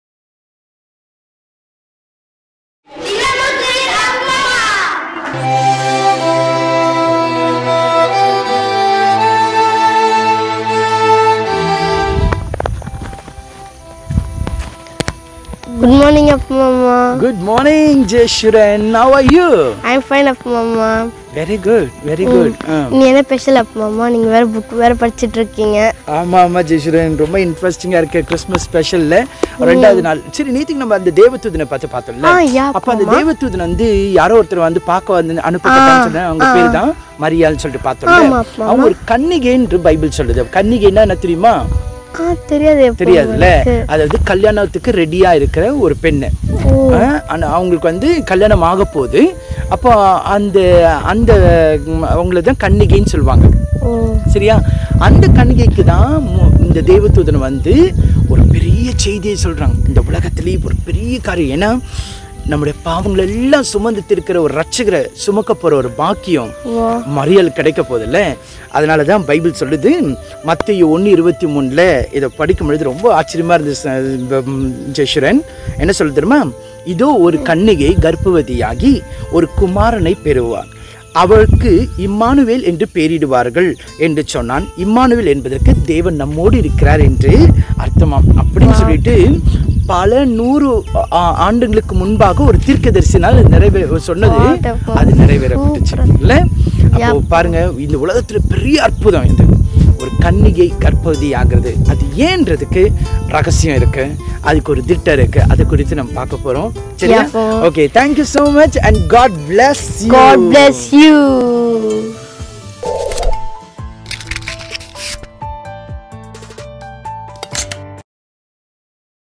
நாள் 2 : இன்றைய முத்து - ஒரு கன்னிகை கர்ப்பவதியாகி.. | தினமுத்து 2.0 வித் அப்பு மாமா
தினமுத்து (டிடி) என்பது ஆக்கப்பூர்வமான/படைப்பாற்றல் வழியில் வரும் தினசரி வசனம். இப்போது டிசம்பர் மாதமானதால் கிறிஸ்துமஸ் சிறப்பாக டிடி பதிப்பு 2 இல் நிறைய பொதுவான தகவல்கள் மற்றும் வாழ்க்கையுடன் இணைக்கும் பல விஷயங்களுடன் வெளியிடப்பட்டது. ஒவ்வொரு நாளும் இயேசுவின் பிறப்பில் நடந்த ஒரு பொதுவான விஷயத்தை நம் வாழ்க்கையுடன் இணைகது. தமிழ் மற்றும் ஆங்கிலம் டிடி குழந்தைகளுடன் செய்யப்படுகிறது,  குழந்தைகள் அன்புடன் அப்பு மாமா மாமாவுடன் உறையாடுவது கண்டுகளிக்க இனிமையாய் உள்ளது.

நாள் 2 : இன்றைய முத்து - ஒரு கன்னிகை கர்ப்பவதியாகி.. | தினமுத்து 2.0 வித் அப்பு மாமா| மத்தேயு 1:23

உங்கள் அனுபவங்களை/ சாட்சிகளை பகிரவும். Comment பண்ணுங்க